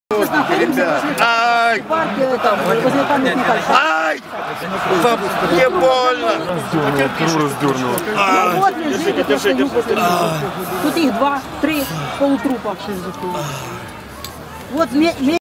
Wounded Man After Clashes in Mykolayiv
At least 10 men were injured during the recent clashes in Mykolayiv. Based on the information coming from "Prestupnosti.net", one man has serious leg fractures and head injures, two men have serious head injuries: one representative of the Right Sector was injured with the police stick and the other one, Denys Plishko, Demokratychnyi Al'yans activist, also received head injuries. On the top of that, one woman was also seen with head injuries.